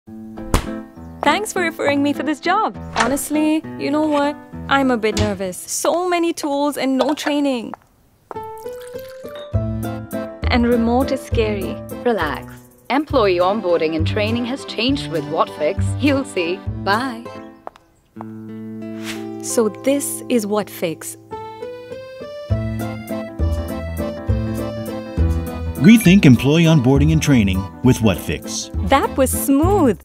Rethink Employee Onboarding and Training with Whatfix
Delight new users with a seamless employee onboarding experience. Whatfix’s interactive on-the-job eLearning solution augments your training by helping employees in learning while doing.